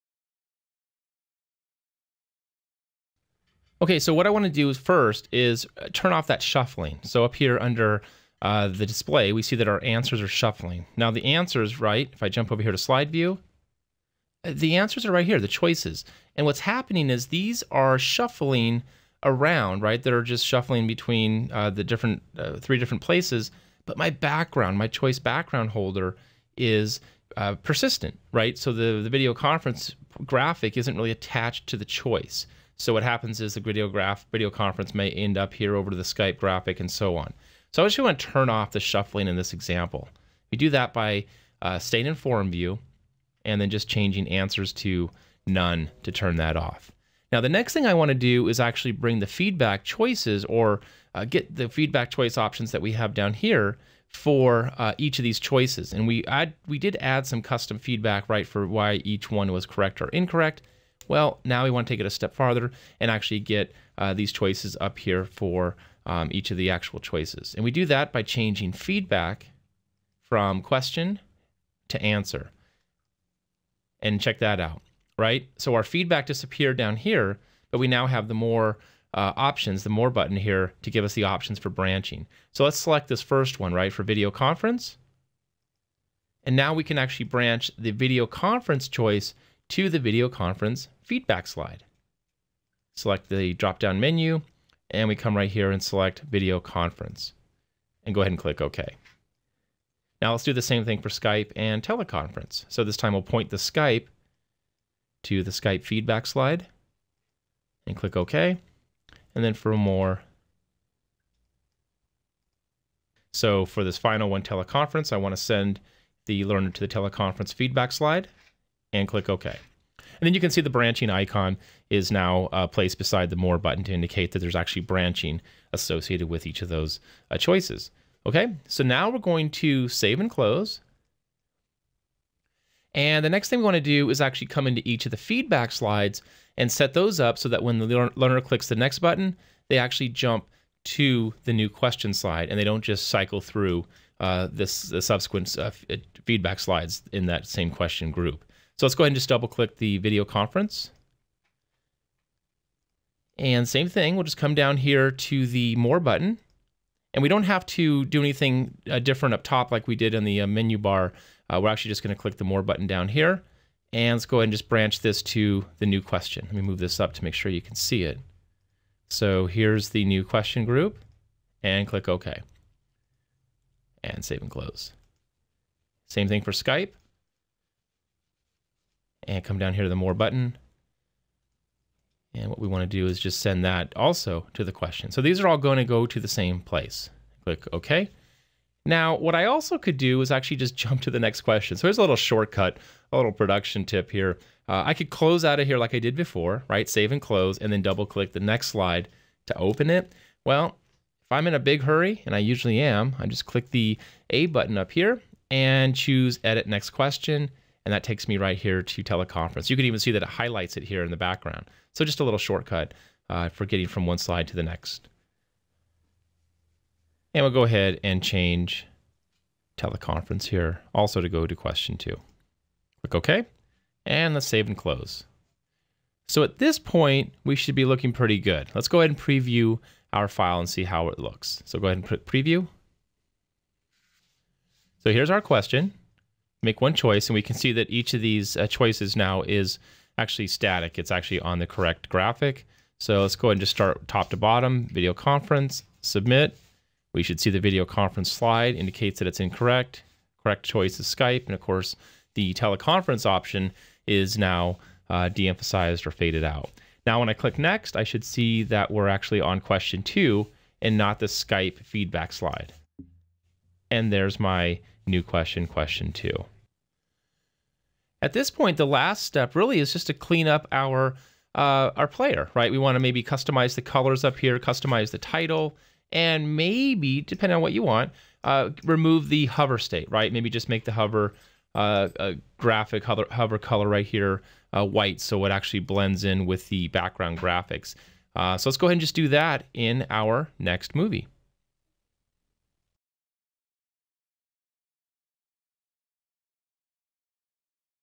Quizmaker Branching Quizzes tutorial: Setting up the branching feedback (8/10)
In this movie, you'll learn how to set up the branching options for your feedback slides.Learn more about this workshop